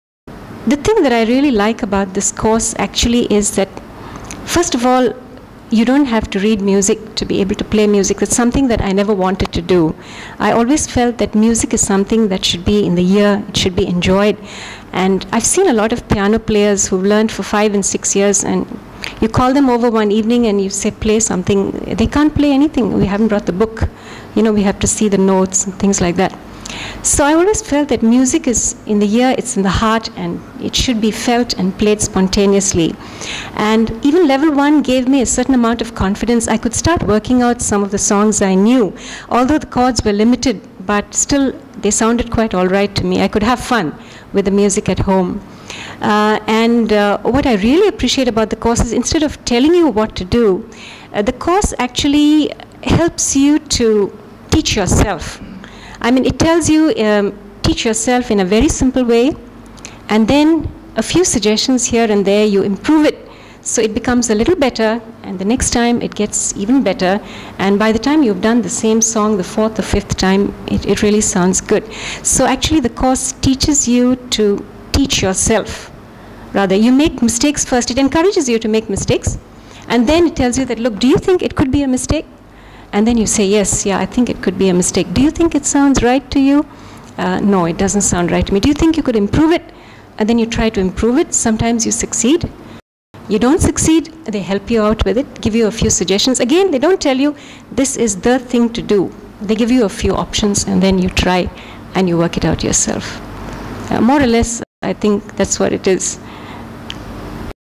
PBE - Dr Gita Play By Ear Student - What she learnt 1/3
Dr Gita was one of our students who graduated in the Play By Ear Course. She was a bit sceptical in the beginning and hesitant with parting the the required fees. She held back for quite a number of months before finally taking the plunge. Boy, was she in for a surprise at the difference it makes. See what she has to say about it in the video. And she spoke to great length as to what it can do for you.